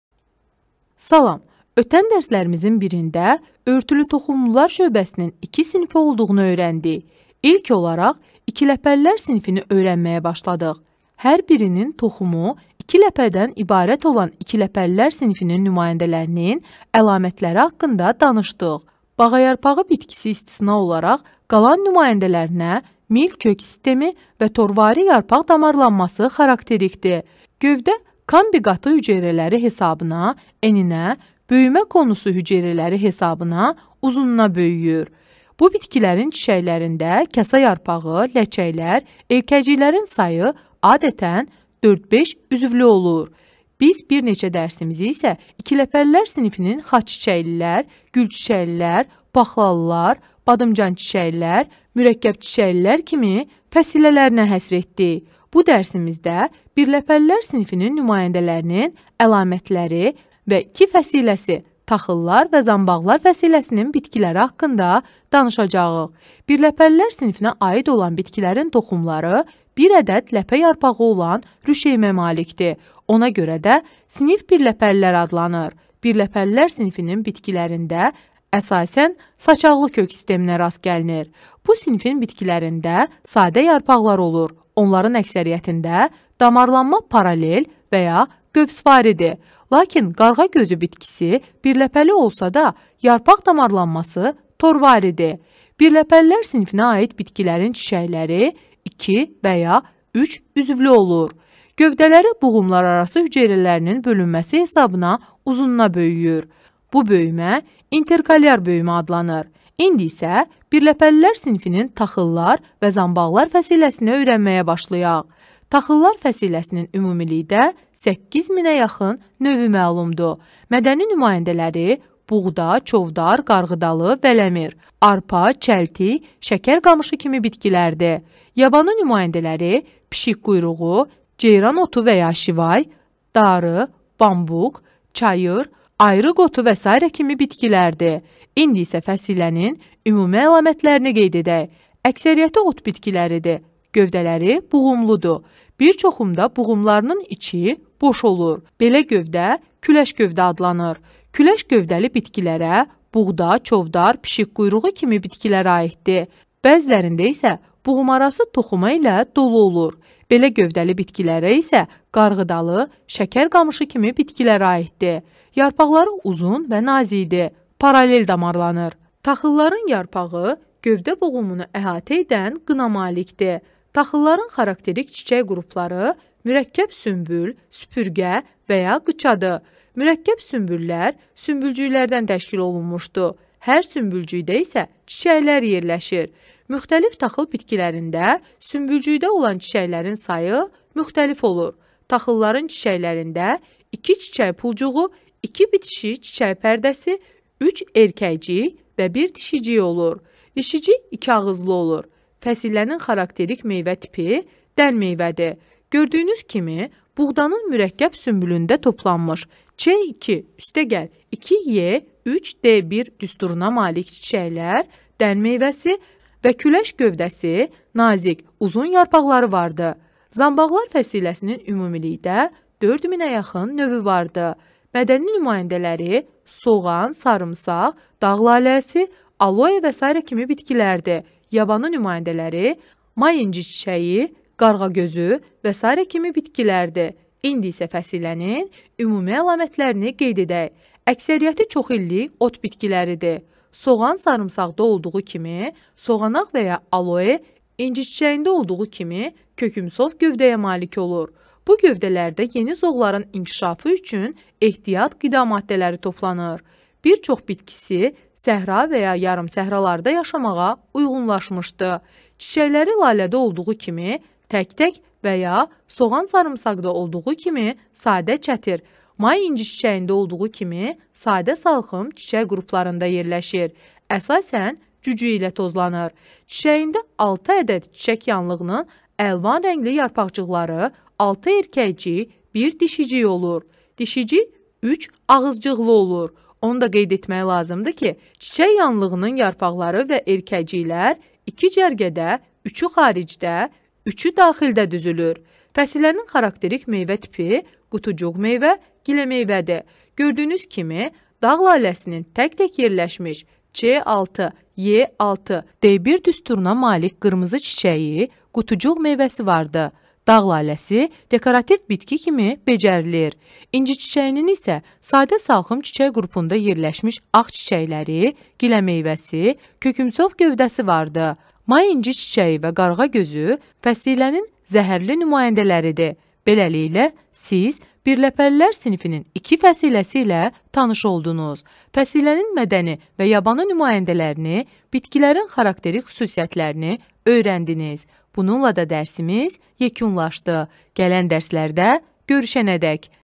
Biologiya 7-ci sinif 18. Birləpəlilər sinfi. Taxıllar və zanbaqlar fəsiləsi.
Müəlliflər: Quliyeva Sevinc Mobil qızı
                   Ömərova Səbinə Novruz qızı